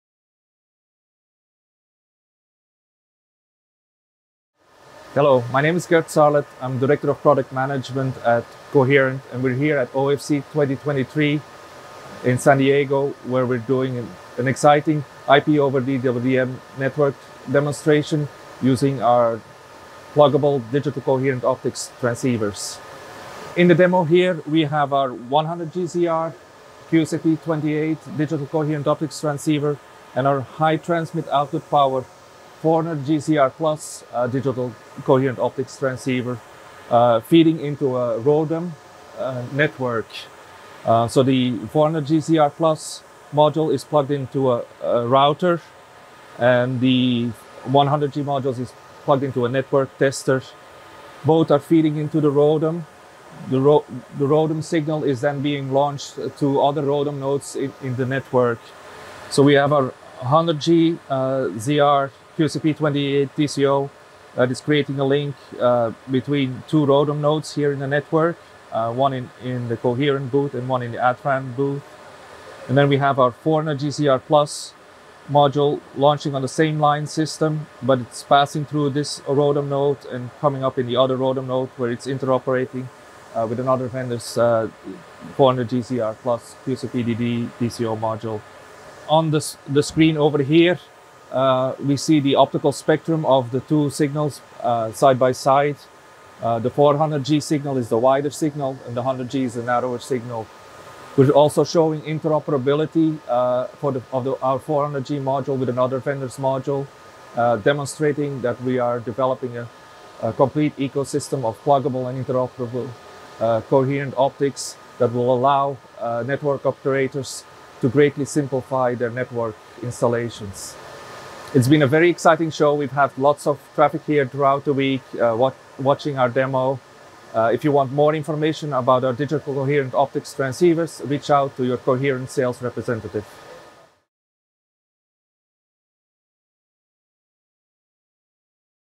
Coherent | IP-over-DWDM Demonstration at OFC 2023
Our transceivers for optical communications will fundamentally change how optical transport networks are deployed, simplifying network architectures by eliminating unnecessary layers of equipment, which can be shown in this demo of our 100ZR QSFP28-DCO and high transmit output power 400G-OpenZR+ QSFP-DD-DCO transceivers feeding a ROADM network.